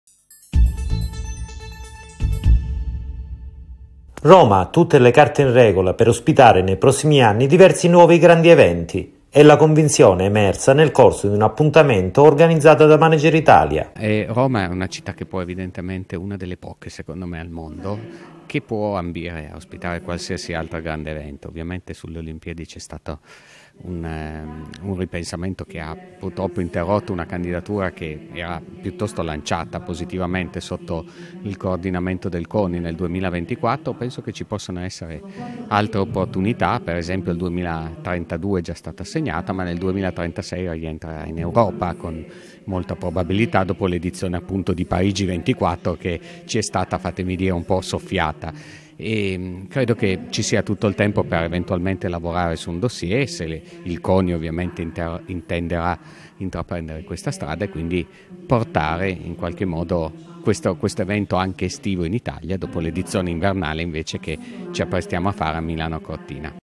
Daneo (WePlan Srl): "Roma perfetta per grandi eventi"
L'intervento di Roberto Daneo all'appuntamento organizzato da Manageritalia